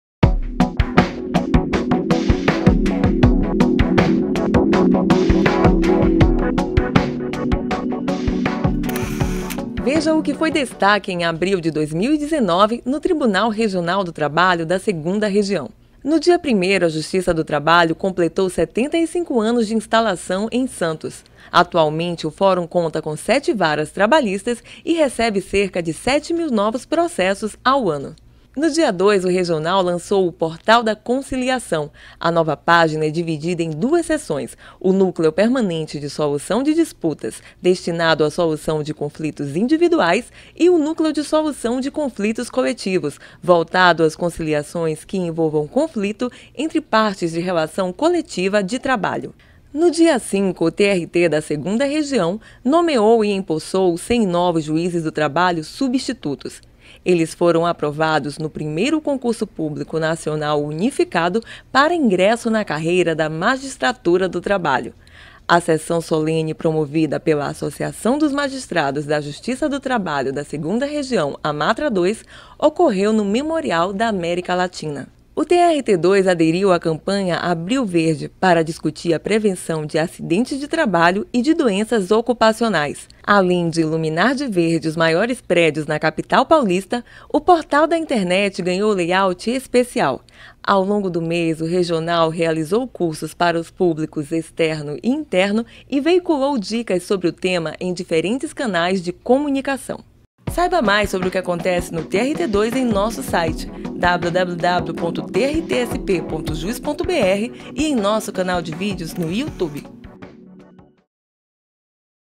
Destaques de abril no Tribunal | TRT em Foco
Lançamento do Portal da Conciliação, aniversário da JT em Santos e Abril Verde. Veja o resumo do mês de abril no TRT-2.

FICHA TÉCNICA:
Texto e narração: Dirlene Pimenta
Edição: Afonso Flores